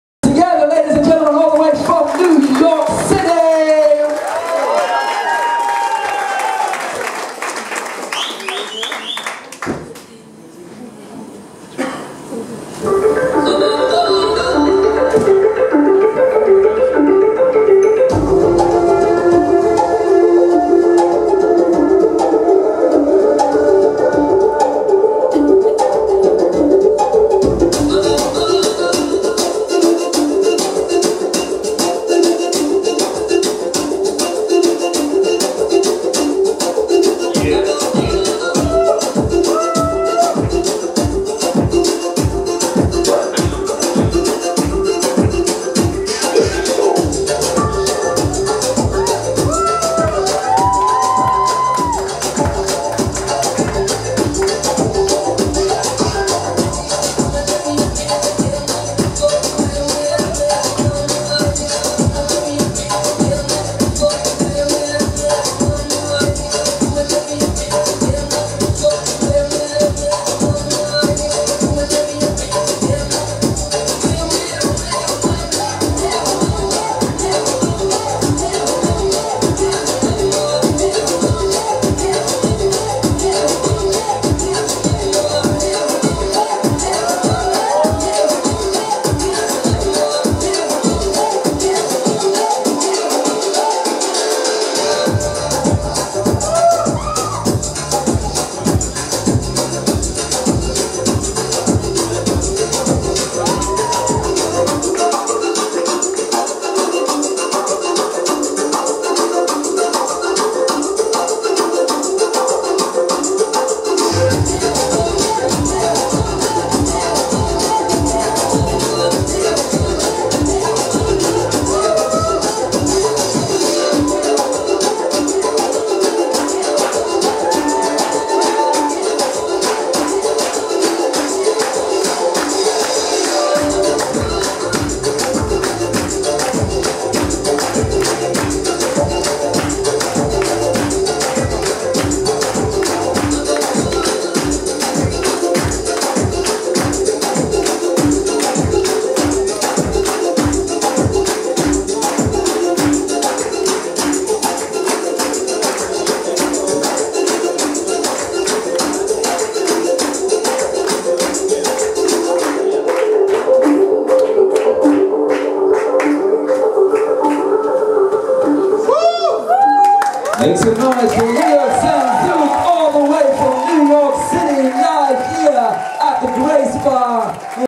Henri Velandia Solo Dance Show, London, UK, May 2015
Henri Velandia (USA)

Solo Dance Show during Henri's visit to London, UK, May 2015.

Location: Grace Bar, Piccadilly Circus

Recorded: ZLDWAmon (Zouk Lambada Distant Worlds)